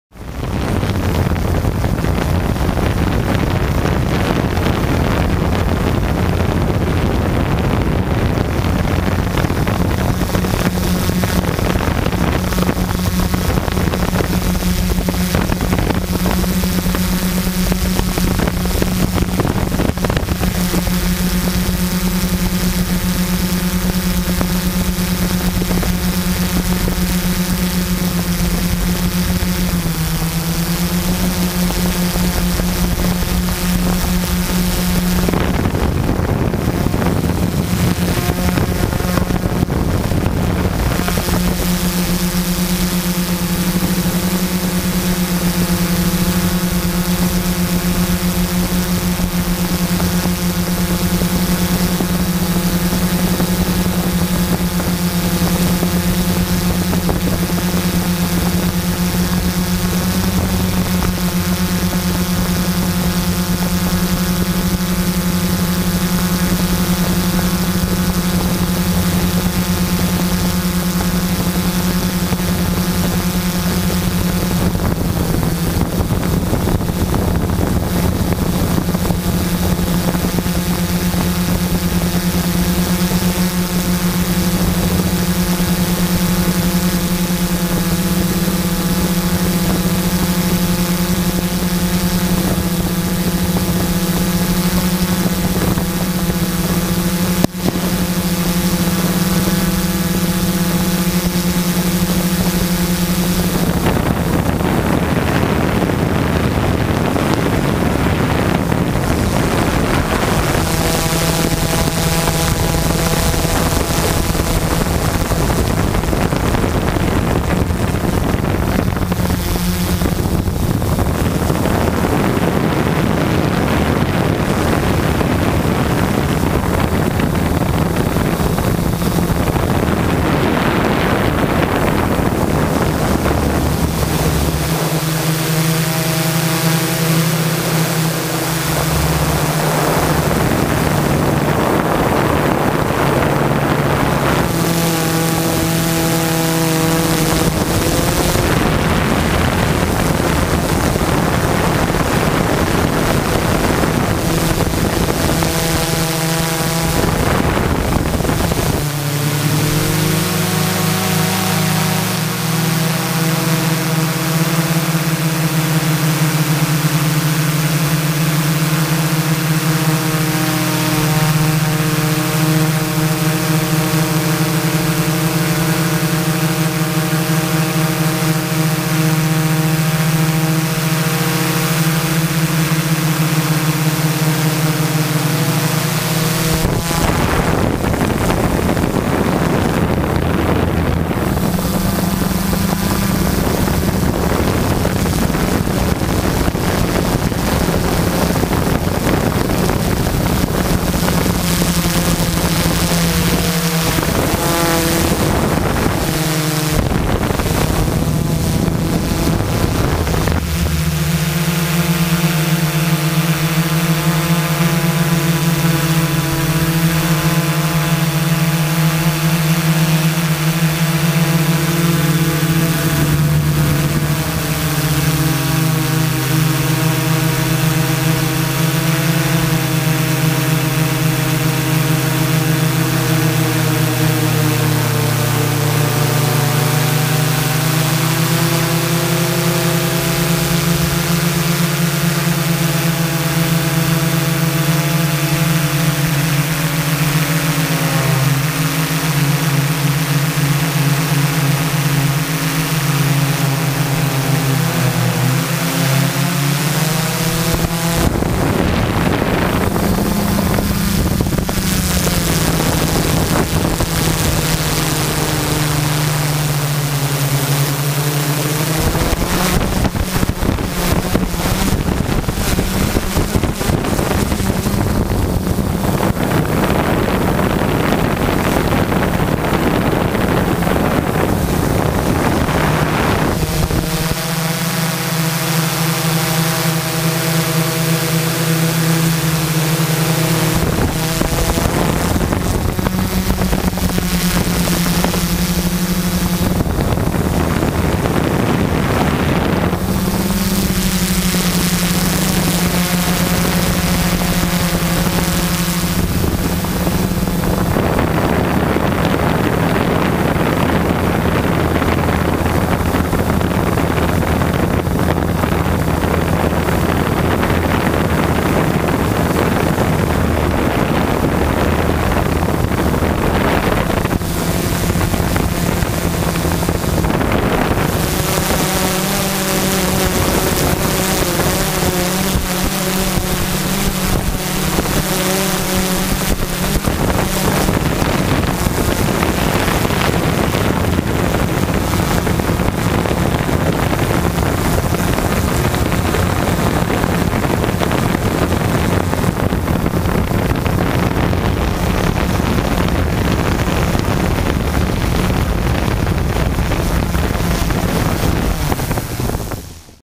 Drones eye view of firework exploding quadcopter GoPro H3+ Black 1242 Self CF Props MultiStar 2212
Here is a view of a canister Excalibur firework exploding from a quadcopter.

The quadcopter contains:

20A Slim Afro Escs (SimonK)
12x42 Self Tightening Carbon Fiber Props (Ebay)
Turnigy 3000Mah 3s 20C
Multistar 2212 920kv
DYS Smart Gimbal
GoPro H3+ Black
X8r Receiver
HML650 Landing Gear

This quadcopter was made relatively cheap but still will be able to lift a GoPro and gimbal.